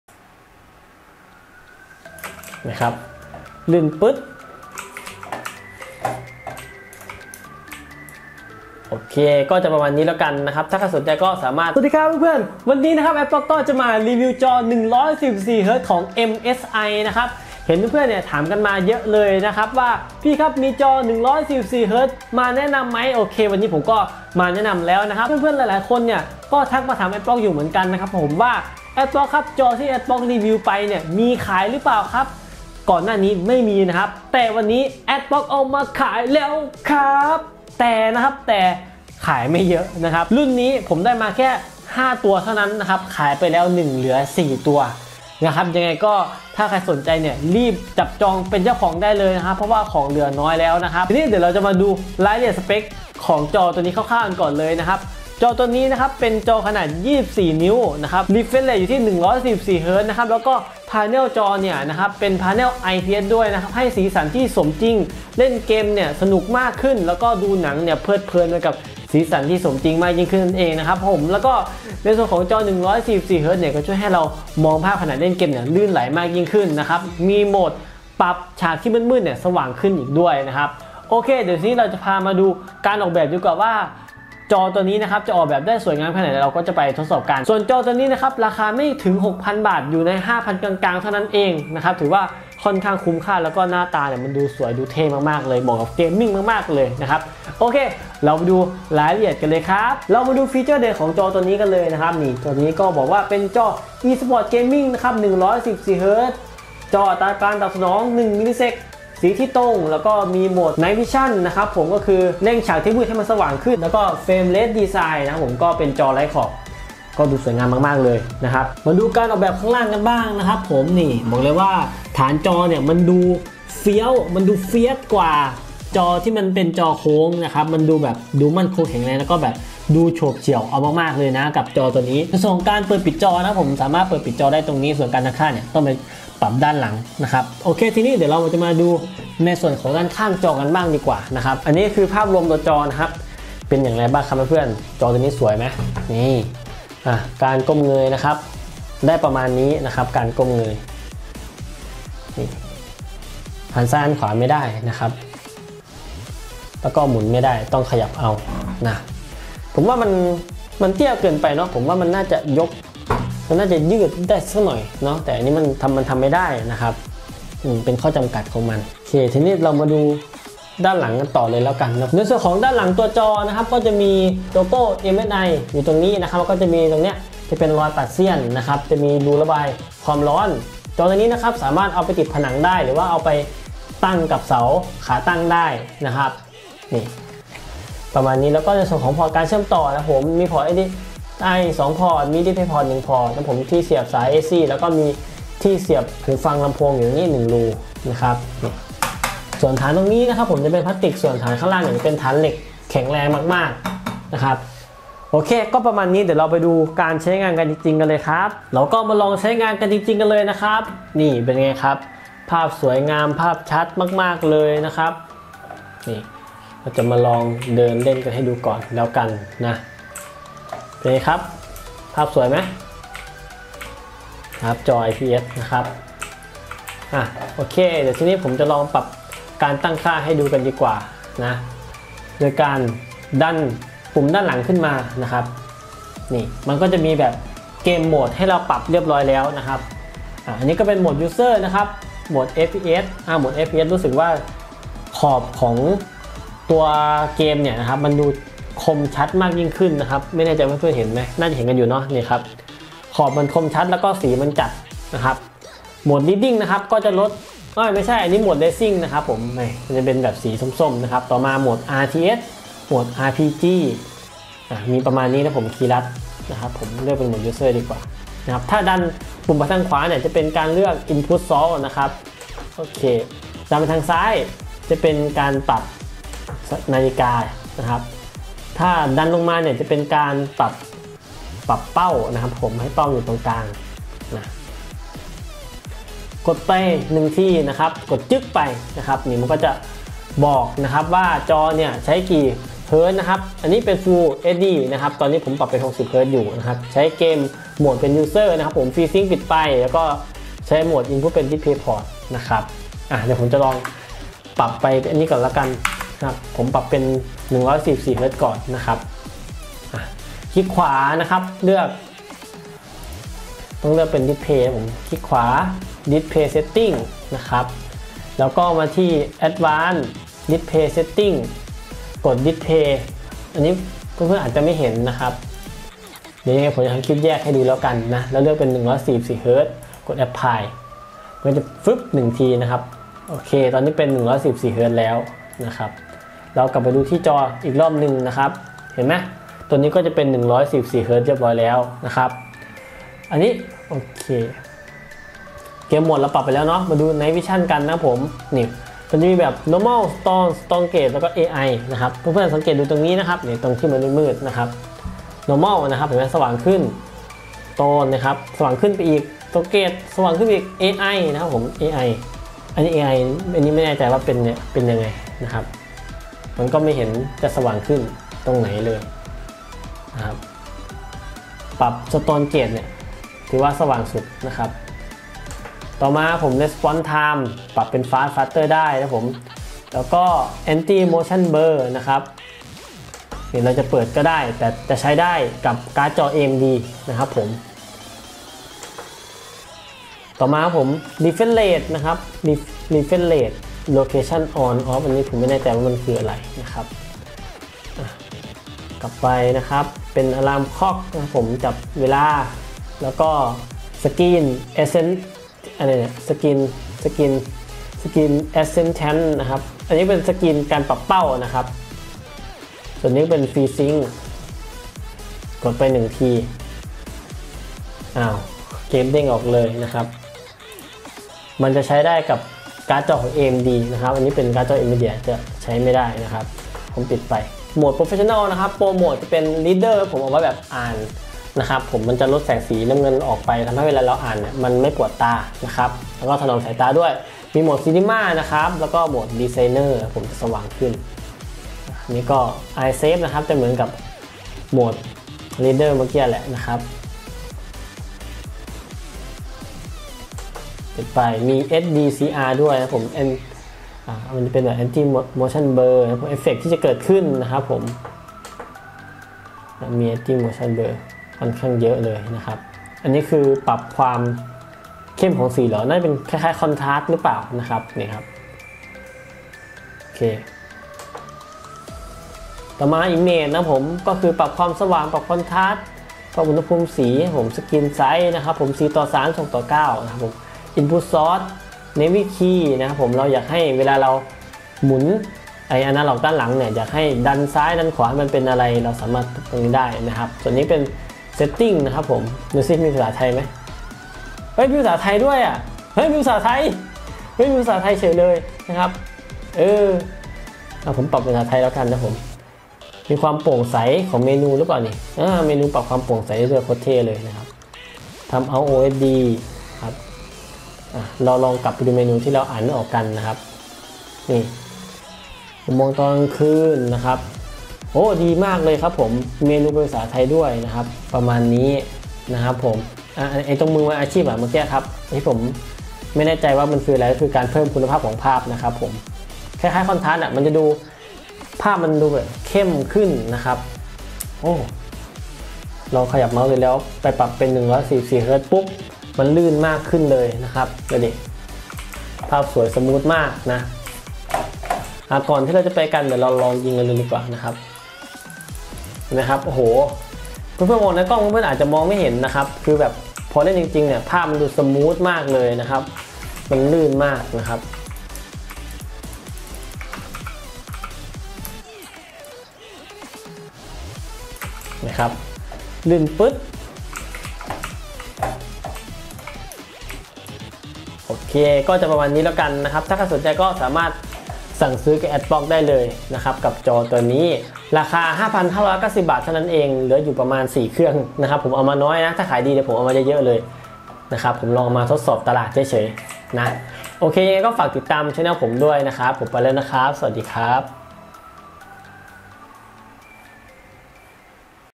รีวิวจอ 144Hz IPS 24 นิ้ว ราคาถูก เล่นเกมลื่น สีสมจริง ขอบจอบาง ถนอมสายตา MSI Optix G241
รีวิวจอ  MSI Optix G241  ราคา 5,590 บาท ประกัน 3 ปี

Monitor 23.8'' MSI Optix G241 (IPS, HDMI, DP) FreeSync 144Hz
-MSI Optix G241ขนาด 23.8” มาพร้อมกับความละเอียด Full HD 1920 x 1080 จะทำให้คุณเพลิดเพลินและสนุกกับการเล่นเกมมากยิ่งขึ้น
-อัตรารีเฟรช 144Hz และเวลาตอบสนอง 1ms ลดความล่าช้าของภาพและภาพเบลอที่เกิดจากการเคลื่อนไหว ช่วยให้คุณเล่นเกมได้อย่างราบรื่น เหนือคู่แข่ง
-AMD FreeSync ป้องกันการฉีกขาดของหน้าจอ ช่วยลดการกระตุกของภาพ ให้ประสบการณ์การเล่นเกมที่ยอดเยี่ยม
-Wide Color Gamut สีและรายละเอียดของเกมจะดูสมจริงและละเอียดยิ่งขึ้น
-ด้วยกรอบจอที่แคบมาก จะทำให้คุณเพลิดเพลินและดื่มด่ำไปกับเกมด้วยการติดตั้งหลายจอภาพได้
-เทคโนโลยี Anti-Flicker และ Less Blue Light ลดปริมาณการสั่นไหวของหน้าจอ ช่วยให้คุณเล่นเกมได้นานขึ้นป้องกันอาการปวดตาและความเหนื่อยล้า
-มีพอร์ตเชื่อมต่อ HDMI และ DisplayPort
-รองรับการติดตั้งกับขาแขวนติดผนัง